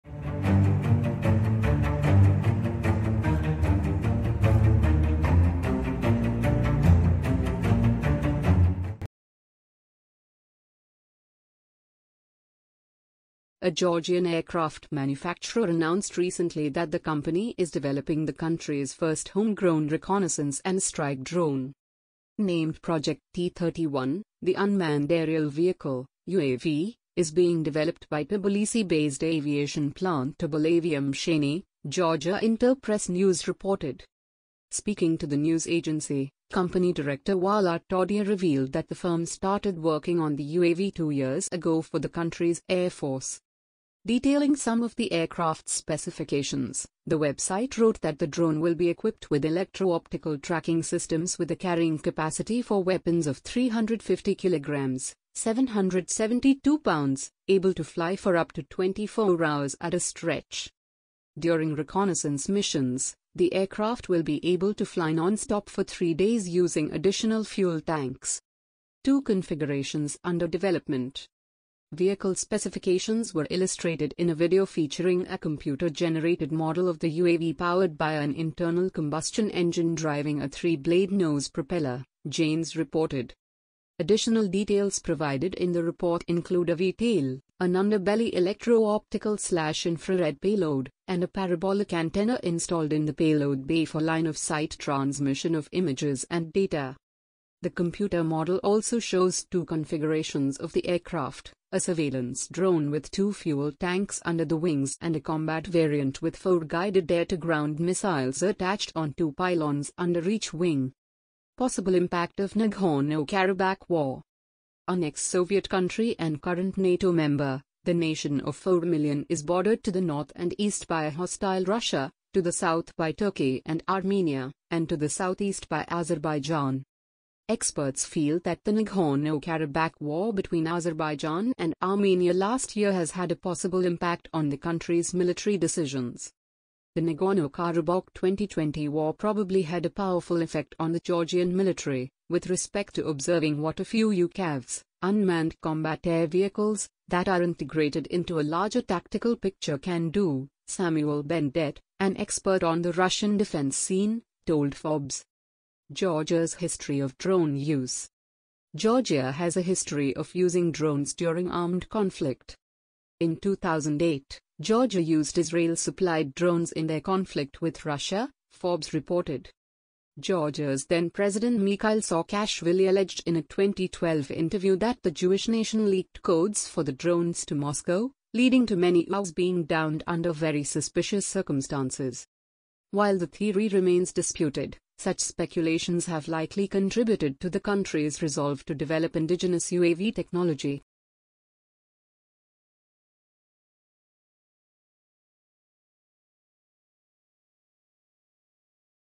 Georgian Aircraft Manufacturer Announces Country’s First Combat Drone
A Georgian aircraft manufacturer announced recently that the company is developing the country’s first home-grown reconnaissance and strike drone.
 Named Project T-31, the unmanned aerial vehicle (UAV) is being developed by Tbilisi-based aviation plant TbilAviaMsheni, Georgia InterPressNews reported.
 During reconnaissance missions, the aircraft will be able to fly non-stop for three days using additional fuel tanks.
 Experts feel that the Nagorno Karabakh war between Azerbaijan and Armenia last year has had a possible impact on the country’s military decisions.
 Georgia’s History of Drone UseGeorgia has a history of using drones during armed conflict.